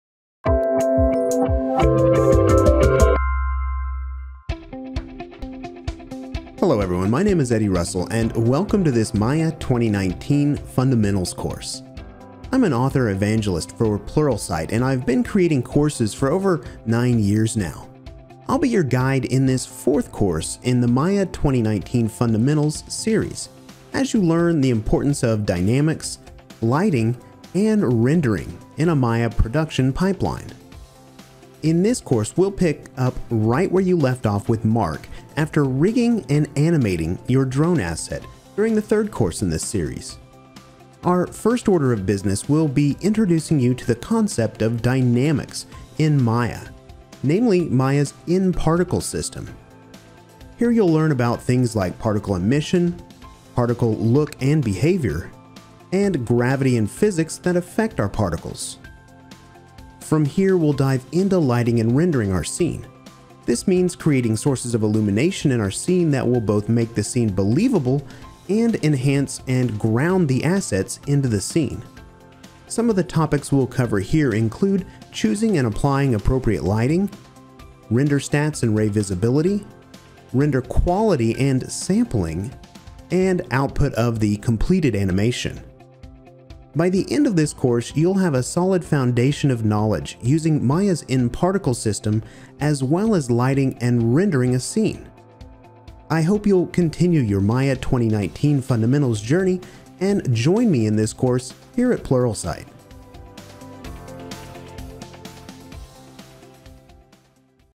Maya Skills: Maya 2019 Fundamentals: Dynamics, Lighting, & Rendering Course Preview
Join Pluralsight author Eddie Russel as he walks you through a preview of his "Maya 2019 Fundamentals: Dynamics, Lighting, and Rendering" course found only on Pluralsight.com. Build your technology skills with Eddie's help by learning how to choose and apply appropriate lighting, how to render stats and ray visibility, and much more. Visit Pluralsight.com to start learning Maya skills today!